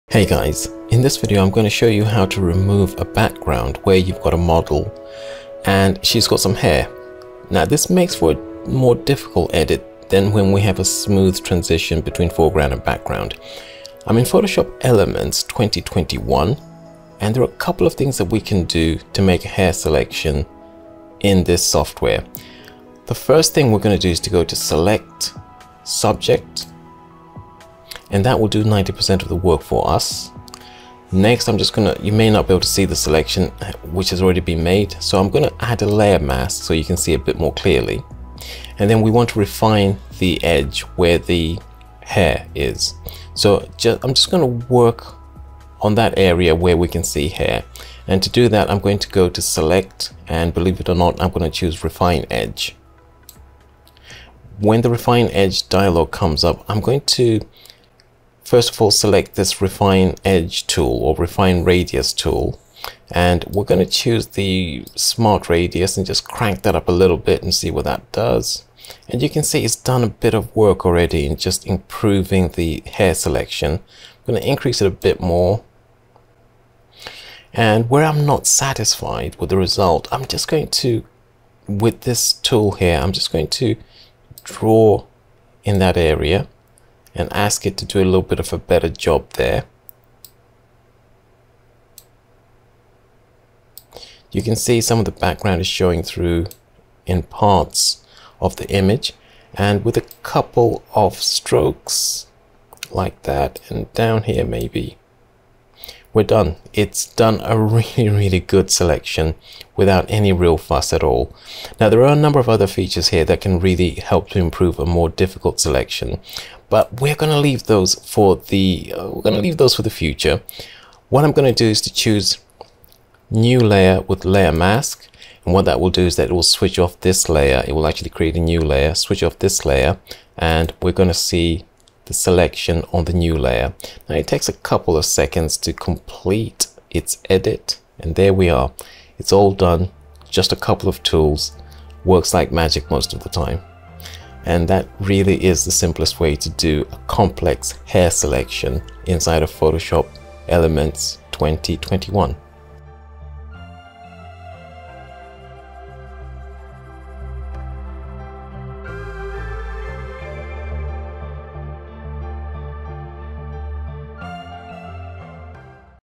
Crop Hair in Two Minutes in Photoshop Elements 2021 Using Select Subject and Refine Edge
Photoshop Elements has a couple of tricks for selecting hair and removing backgrounds.  In this video we take a look at the AI enhanced features that make this a straightforward task for retouchers and designers.

HARDWARE USED
Graphics card - Nvidia RTX 3070
CPU - AMD Ryzen mid range